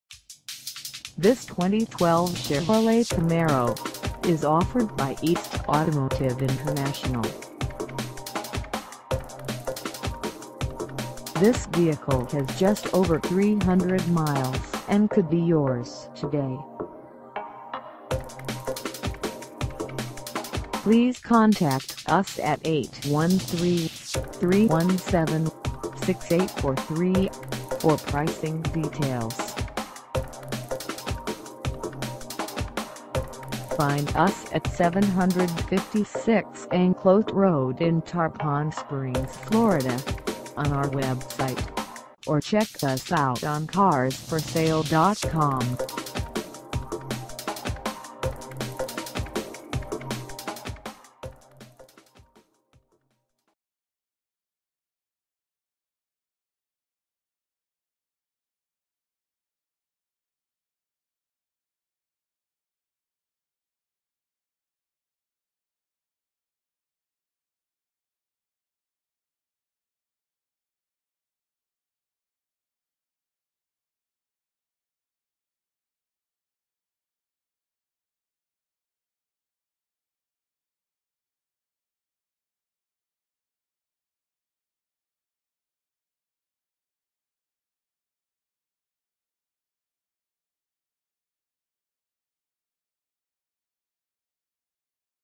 2012 Chevrolet Camaro - for sale in Tarpon Springs, FL 34689
E Automotive International
756 Anclote Road

Come test dirve this 2012 Chevrolet Camaro  for sale in Tarpon Springs, FL.

What more can you say it's a ZL1! Stock photos shown. Taking offers. Cars will be here soon! BLACK, RED, or GREY Exterior Colors Available. Interiors - Black Leather with Red Stitching. 580 hp/556 lb ft torque. Very limited production. Car is loaded with standard features. All Cars ordered with: 6-speed Manual Transmission. 20-in Bright (polished) Aluminum Wheels. Power Sunroof. Exposed-weave Carbon Fiber Hood Insert. Suede Package on Steering Wheel, Shift knob, and Shift Boot. Loaded!

Coming Soon!!!

There is too much to list. Everything about this BEAST is AWESOME.

Please call if you have any questions or would like to make an offer.